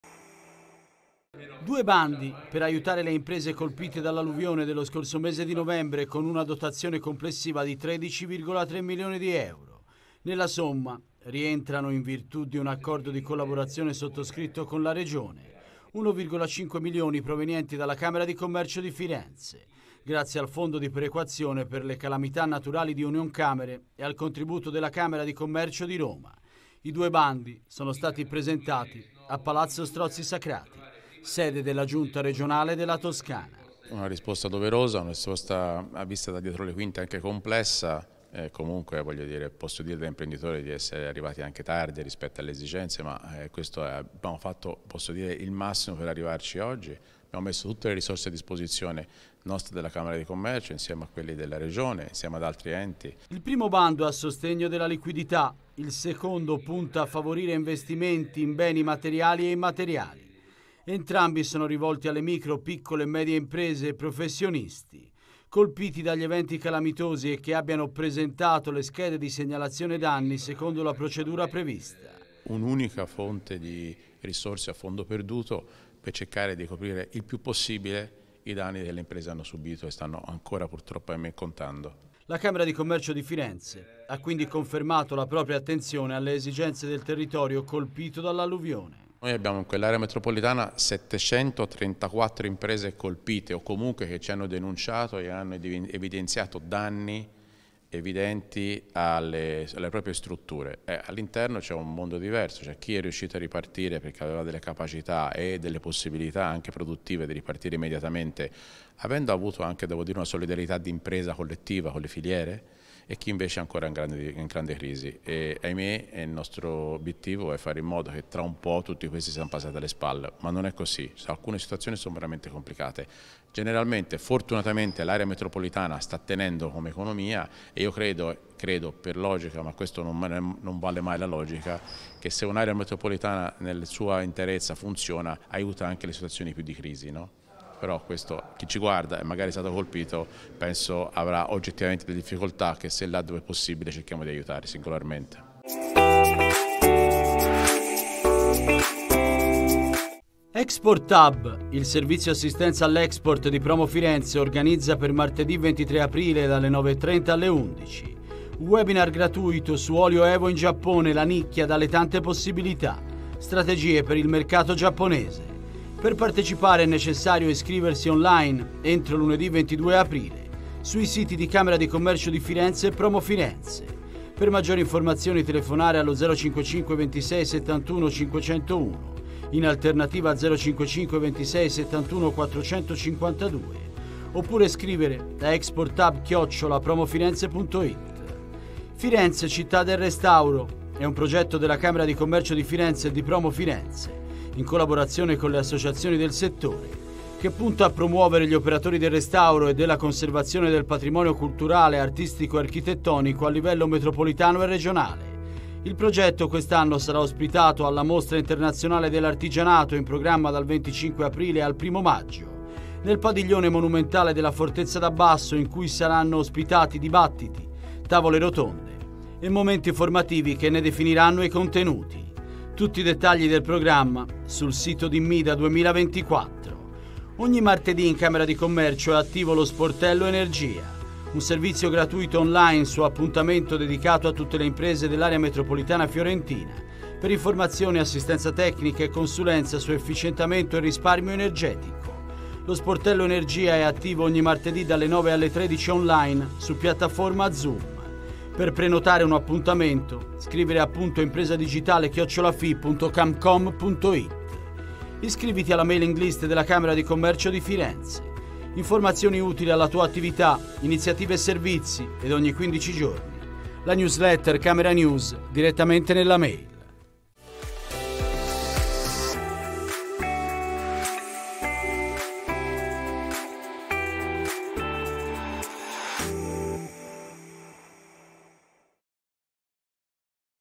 Casa delle imprese 13a puntata 19 aprile 2024 - Nuove risorse per le aziende alluvionate
Due bandi per aiutare le imprese colpite dall'alluvione dello scorso mese di novembre con una dotazione complessiva di 10,3 milioni di euro. Nella somma rientrano, in virtù di un accordo di
collaborazione sottoscritto con la Regione,  1,5 milioni provenienti dalla Camera di commercio di Firenze, grazie al fondo di perequazione per le calamità naturali di Unioncamere e al contributo
della Camera di Commercio di Roma. I due bandi sono stati presentati a Palazzo Strozzi sacrati sede della Giunta regionale della Toscana.
Intervista a leonardo Bassilichi, presidente della Camera di commercio di Firenze